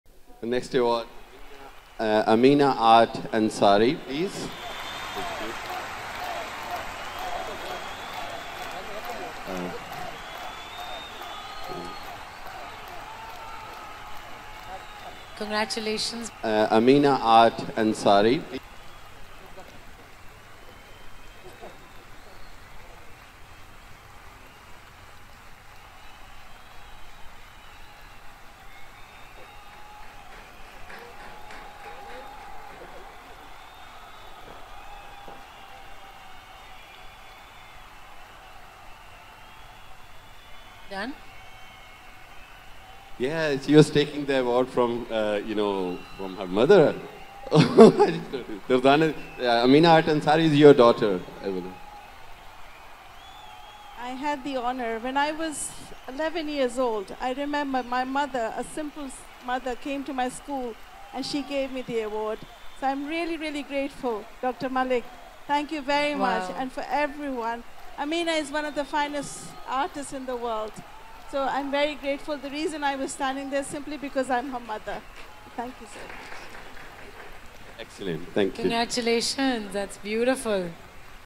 Amina Art Ansari | Martin Edwards M.U.F.C | Irfan Malik CEO | Easy E-Money | Awards | Aug 2023
Ms. Amina Art Ansari received lifetime achievement award from Martin Edwards, President of Manchester United Football Club & Irfan Malik CEO of Easy E-Money at Awards ceremony, Aug 2023 at Bolton Stadium, Bolton.